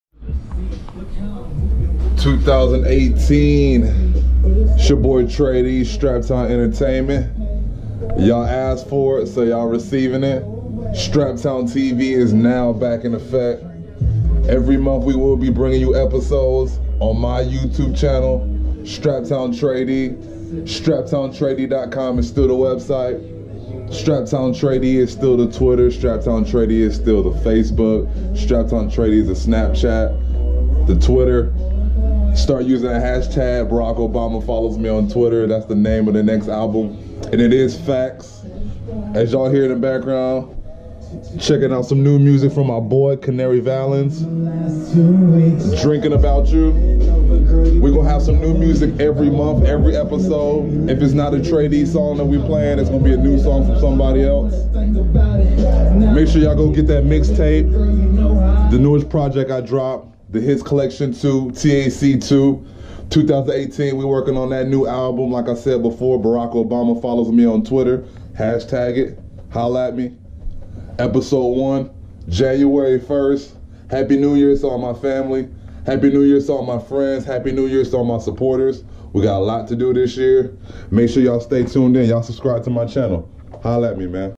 StraptownTV is back
Tray-D announces the return of StraptownTV